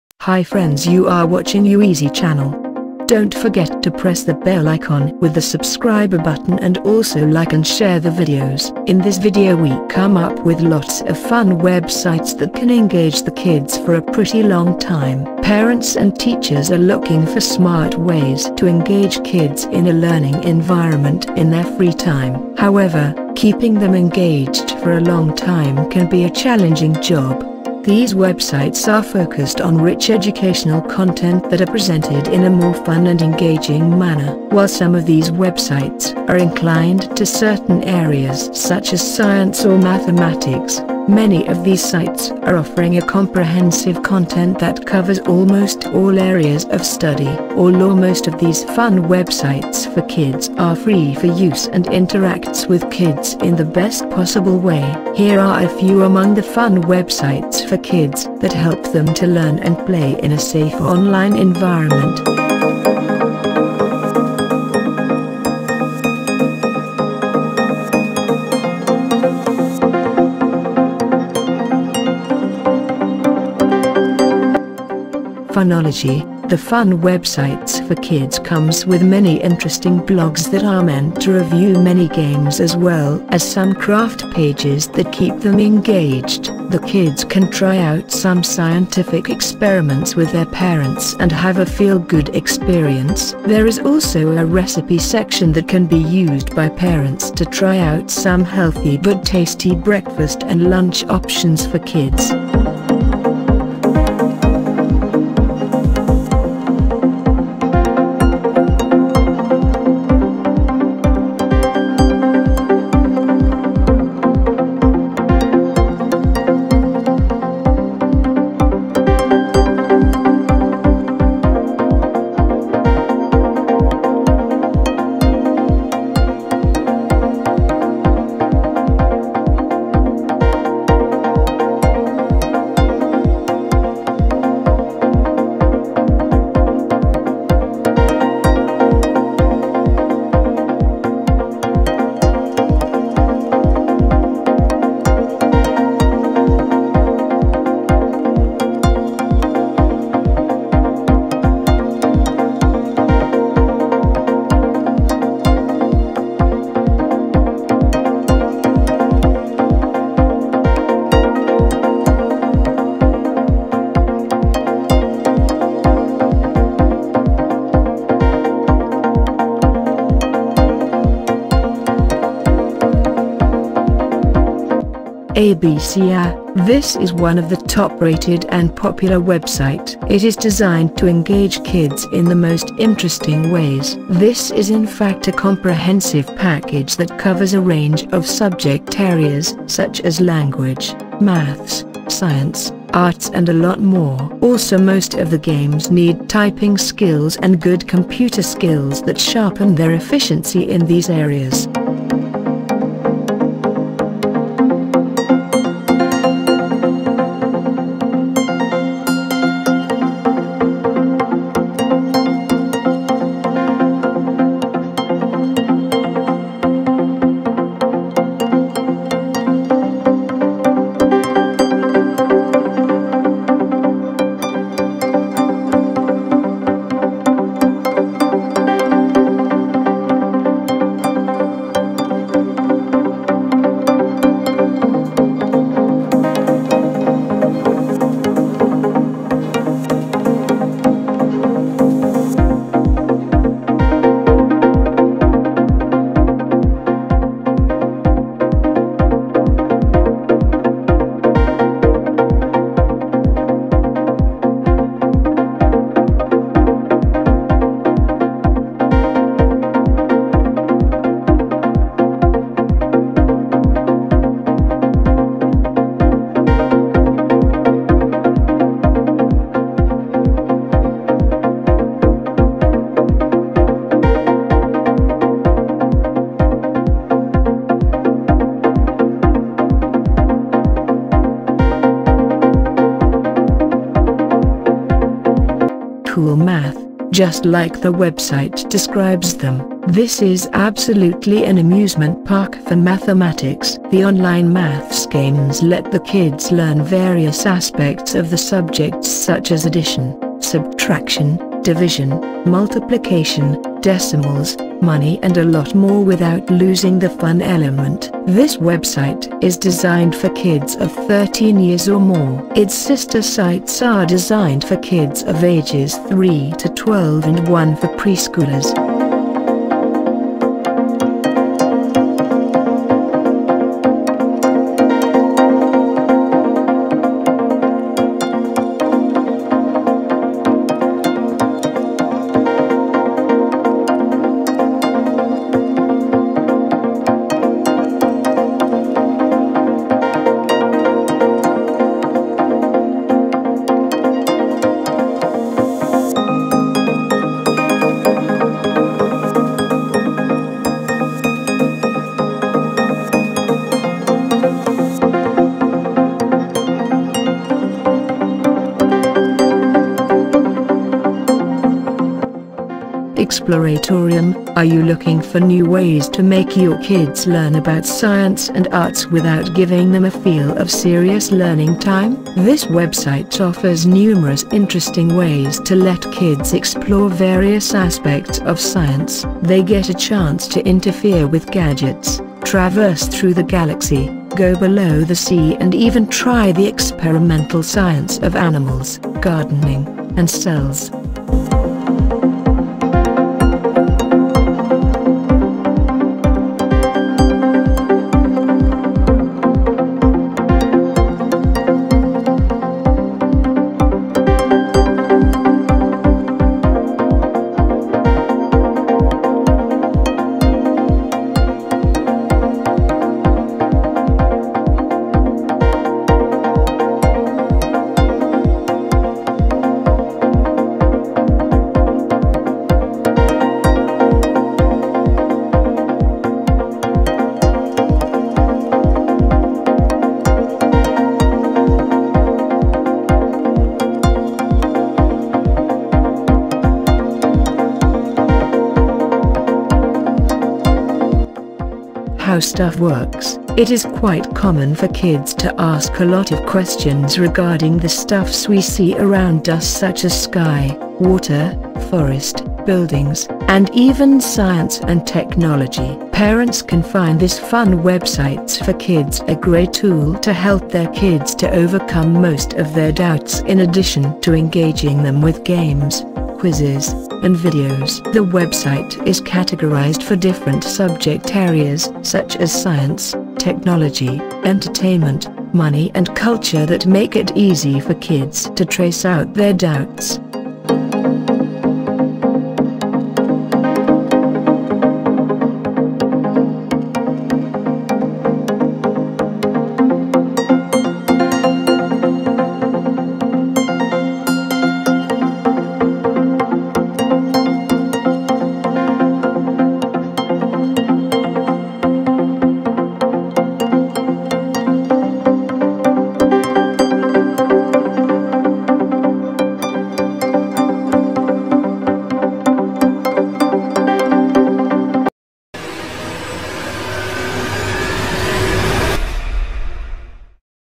Websites for KIDS EDUCATION || YOU EASY
websites for kids education.
Parents and teachers are looking for smart ways to engage kids in a learning environment in their free time.
However, keeping them engaged for a long time can be a challenging job.
These websites are focused on rich educational content that are presented in a more fun and engaging manner.
While some of these  websites are inclined to certain areas such as science or mathematics, many of these sites are offering a comprehensive content that covers almost all areas of study.